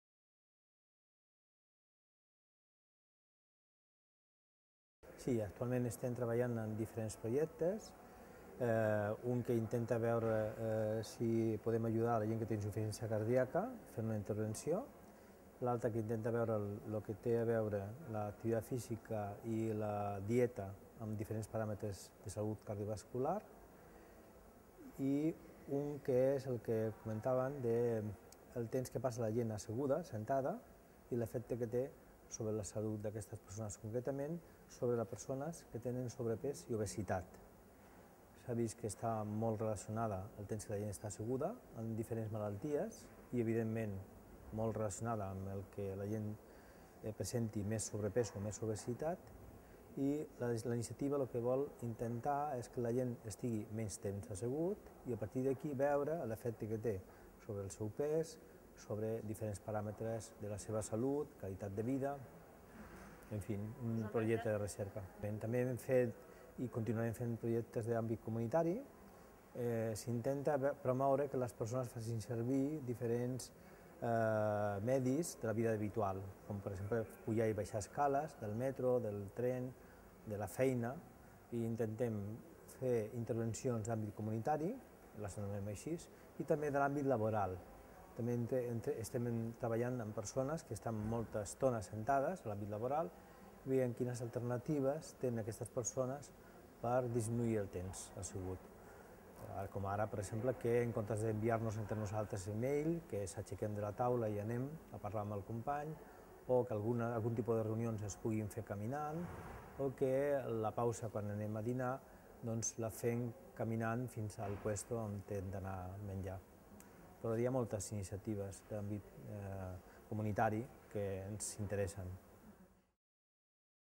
Entrevista Dr. Carlos Martín Cantera
Entrevista al Dr. Carlos Martín Cantera, ponent de "La recerca en activitat física a l'atenció primària: una línia prioritària en la promoció dels estils de vida saludables". Unitat de Suport a la Recerca Barcelona Ciutat de l'Institut d'Investigació en Atenció Primària Jordi Gol.
5a Jornada de Recerca de l'Institut Català de la Salut
Novembre 2012